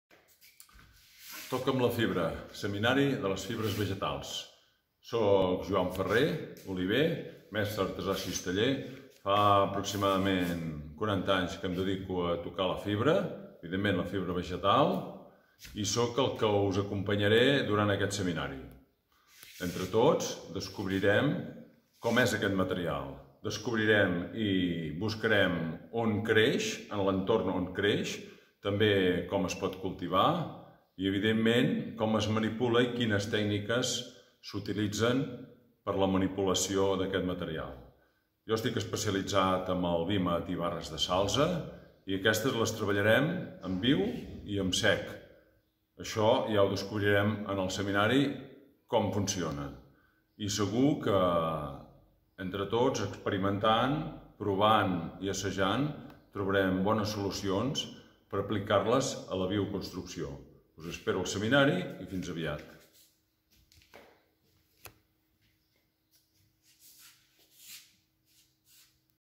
Toca’m la fibra: seminari per a joves professionals
En les fibres naturals s’empren diferents materials i tècniques, el trenat, el teixit o l’entramat, amb materials com el vímet, la pauma , el castanyer i altres fibres vegetals que podem trobar a l’entorn natural.  Els participants del seminari ‘Toca’m la fibra’ coneixeran materials i processos tècnics propis de la cistelleria aplicables a l’arquitectura de la ma del mestre artesà de cistelleria i premi nacional d’artesania Joan Farré, director de l’empresa Pont de Querós.

Aquest seminari vol situar als joves participants en les fronteres disciplinàries, en els límits entre sabers, per explorar com es veuen les coses quan incorporant tècniques i materials de cistelleria (fibres naturals) i es capbussen en l’experimentació fent possible la confluència i l’alternança de mirades diferents, però complementàries.